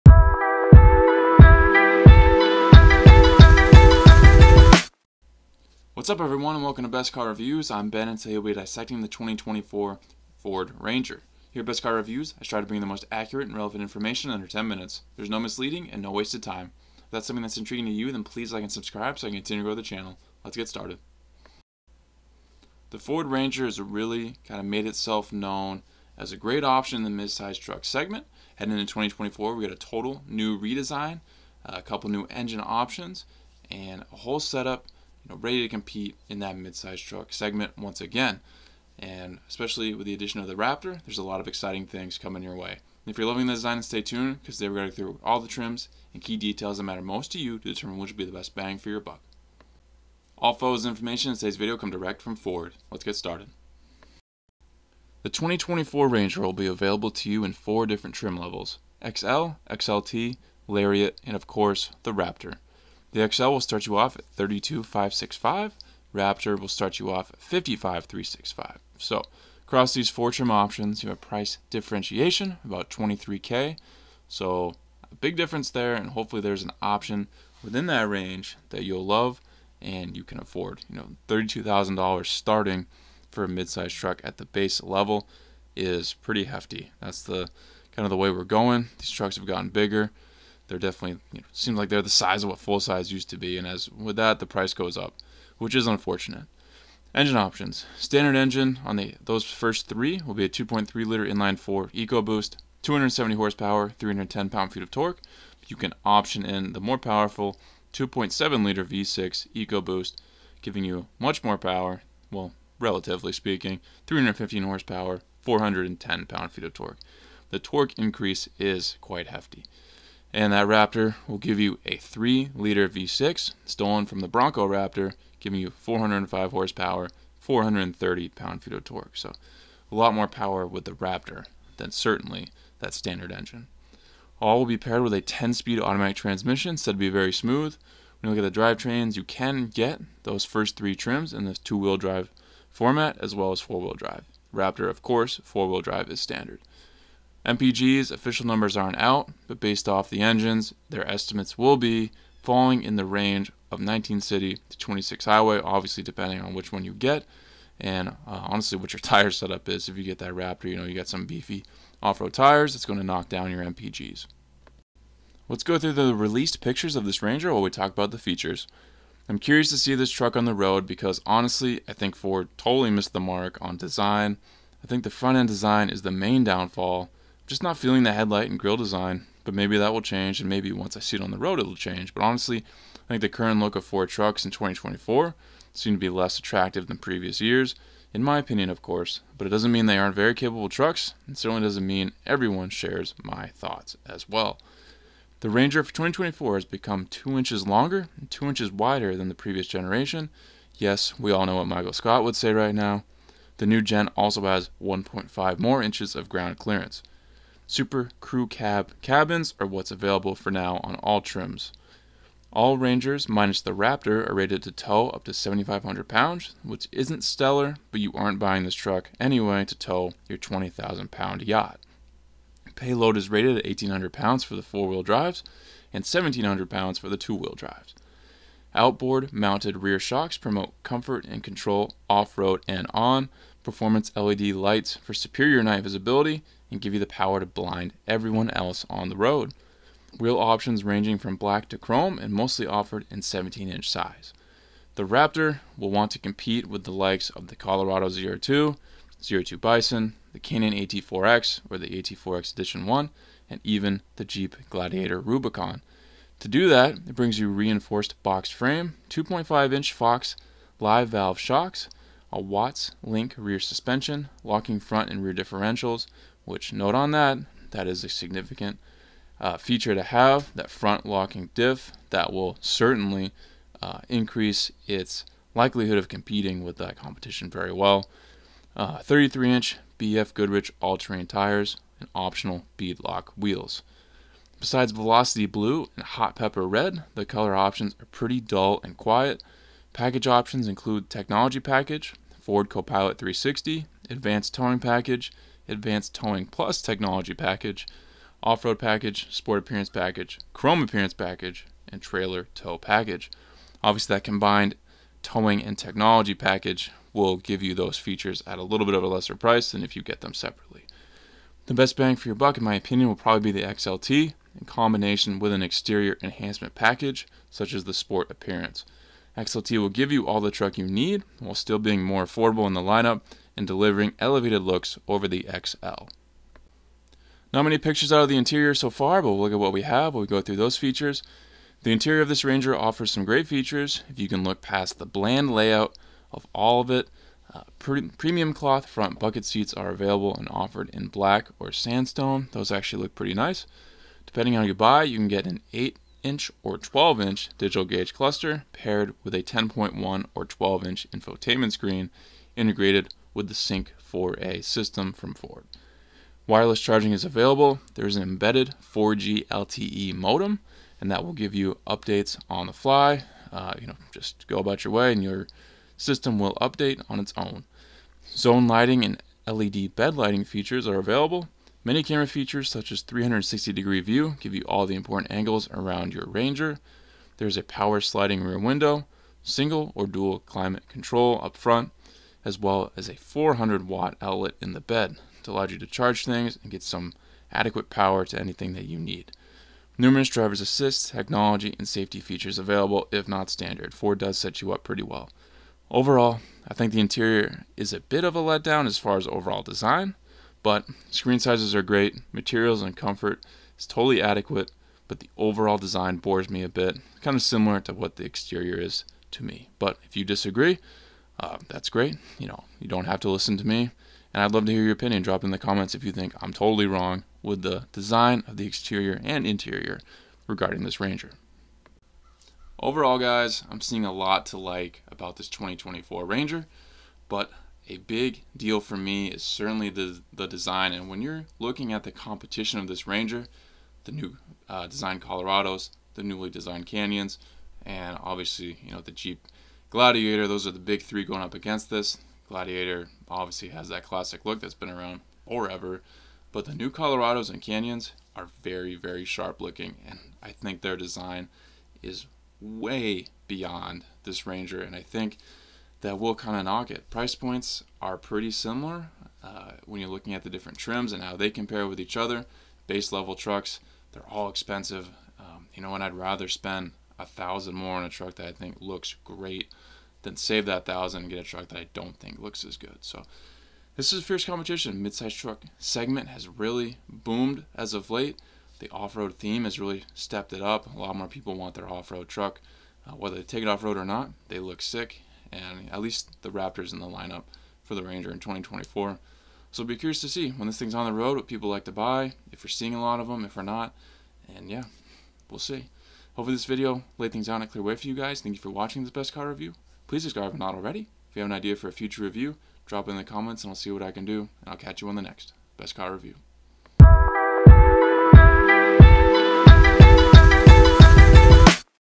2024 Ford Ranger: Trims, Key Features, and More!
Learn the trims and key details of the 2024 Ford Ranger in this review! Including the Ranger Raptor! In this video I compile the most important available details. Photos and information direct from Ford.